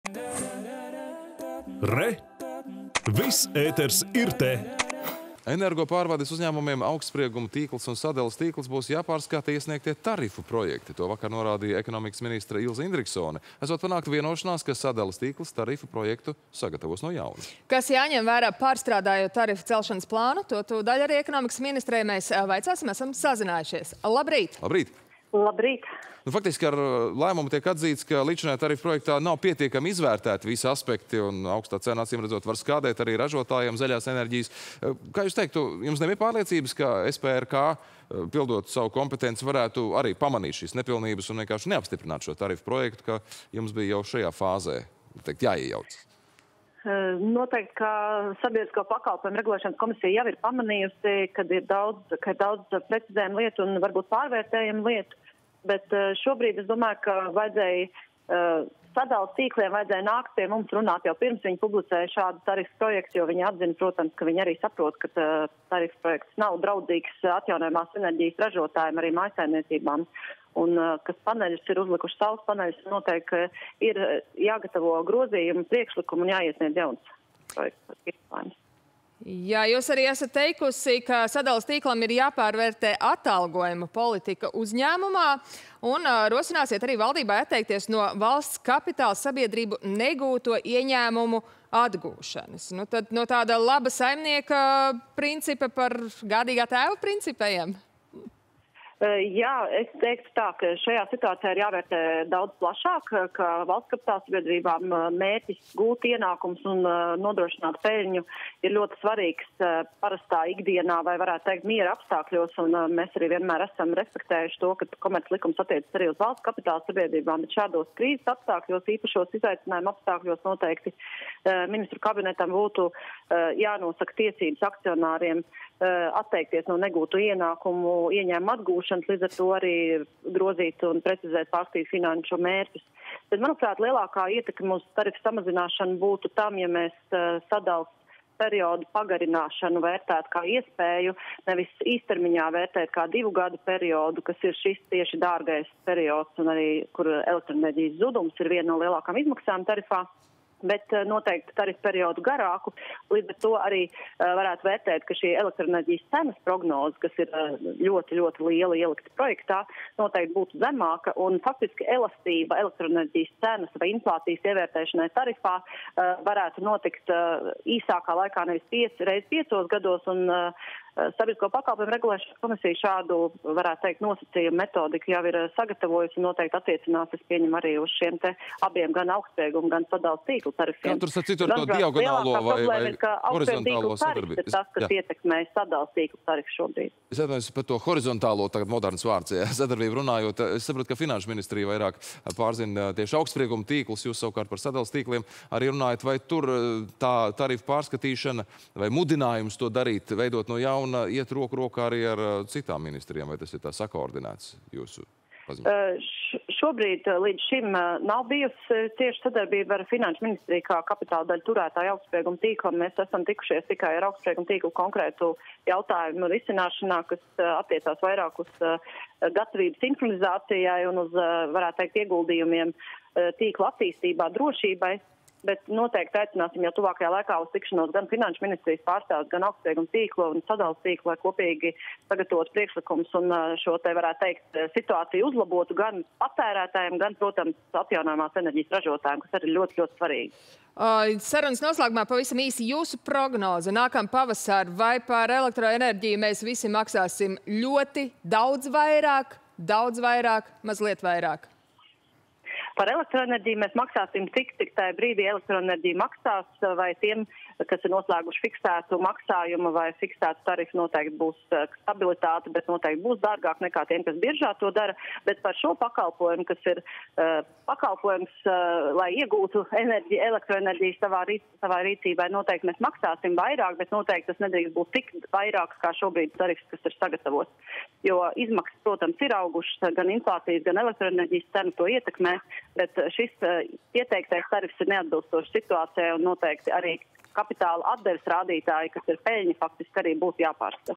Telefonintervija ar ekonomikas ministri Ilzi Indriksoni
Energopārvades uzņēmumiem "Augstsprieguma tīkls" un "Sadales tīkls" būs jāpārskata iesniegtie tarifu projekti, - to vakar norādīja ekonomikas ministre Ilze Indriksone. Esot panākta vienošanās, ka „Sadales tīkls” tarifu projektu sagatavos no jauna.
Kas jāņem vērā, pārstrādājot tarifu celšanas plānu – to ekonomikas ministrei tūlīt vaicāsim, esam sazinājušies telefoniski.